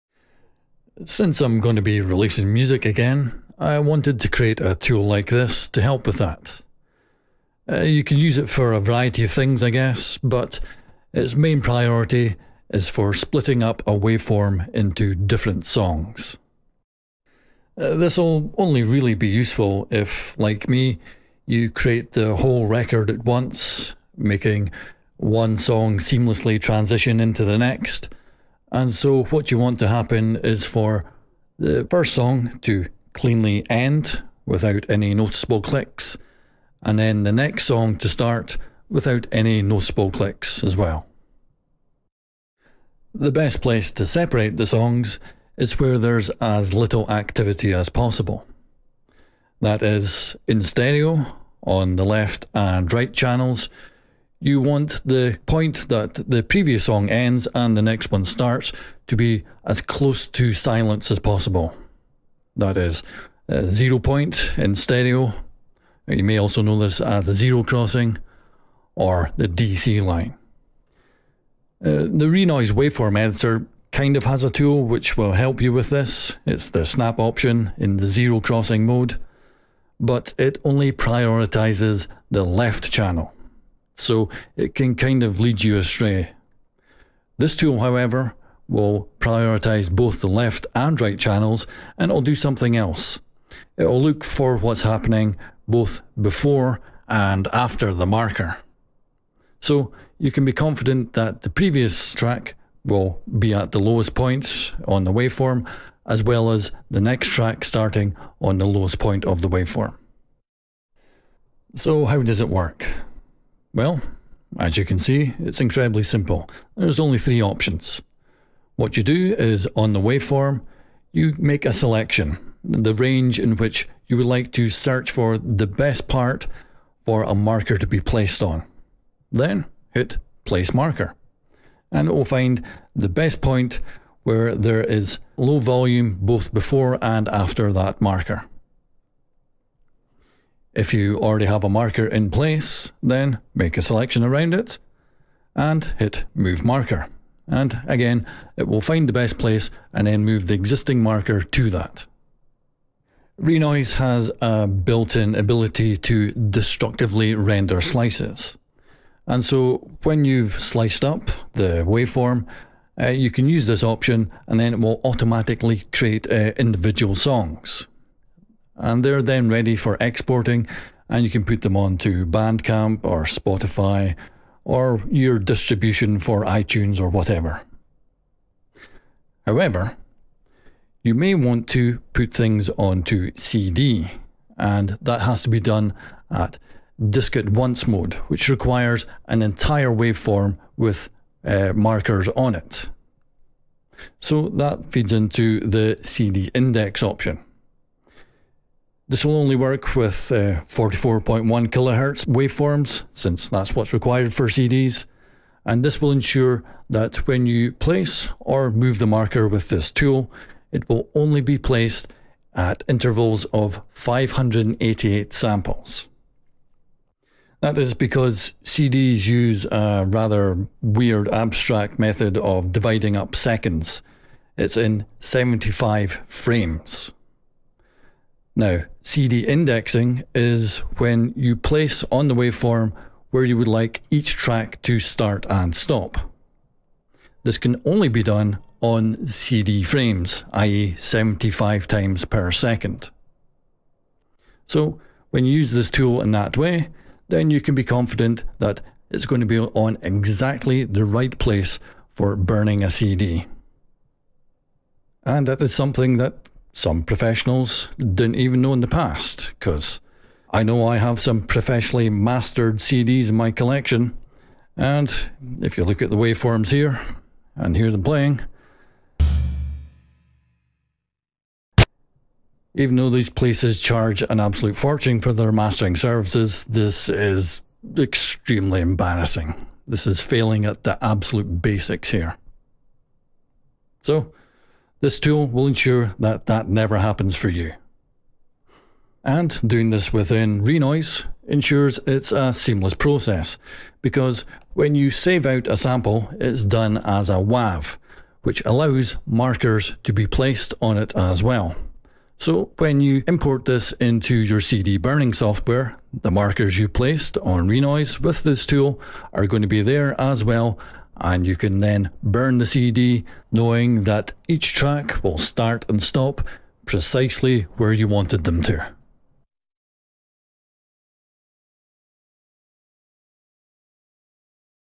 Zero Point Stereo - Renoise Tool
This tool places and moves markers to the best zero point on a waveform. Mainly used for separating songs on the waveform of a full album/EP. Includes an option to pinpoint CD index frames.

0:00 - Intro
1:51 - How The Tool Works
2:59 - CD Indexing
4:14 - Don't Just Split CD Songs Wherever You Like
4:43 - A Seamless Process From Renoise To CD